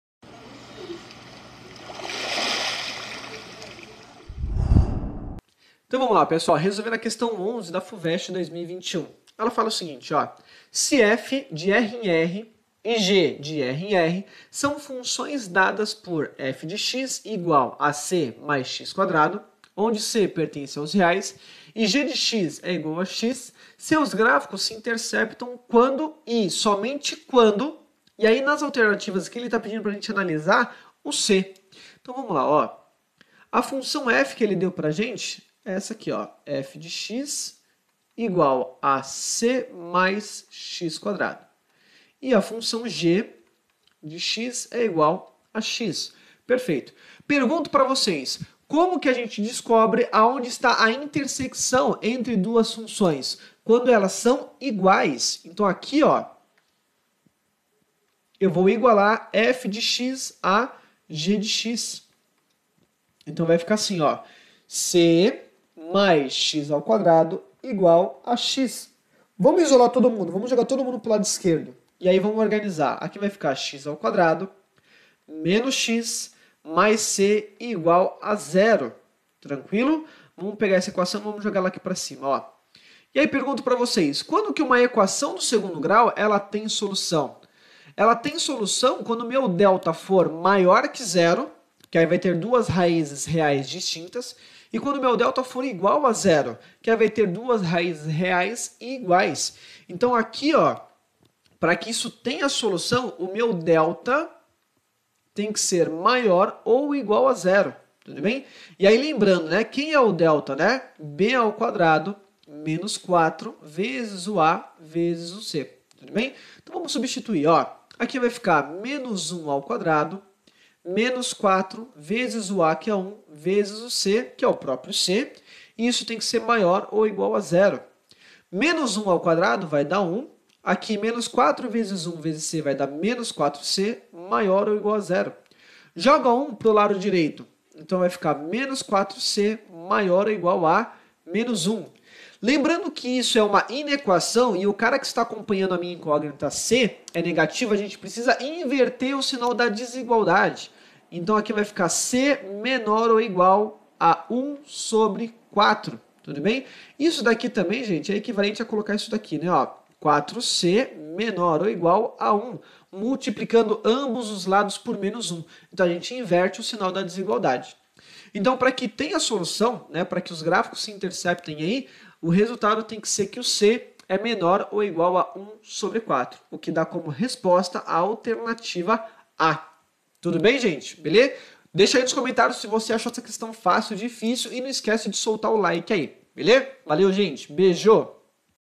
QUESTÃO 11 FUVEST 2021 - Funções
Resolução da questão 11 da FUVEST 2021, de forma clara e objetiva.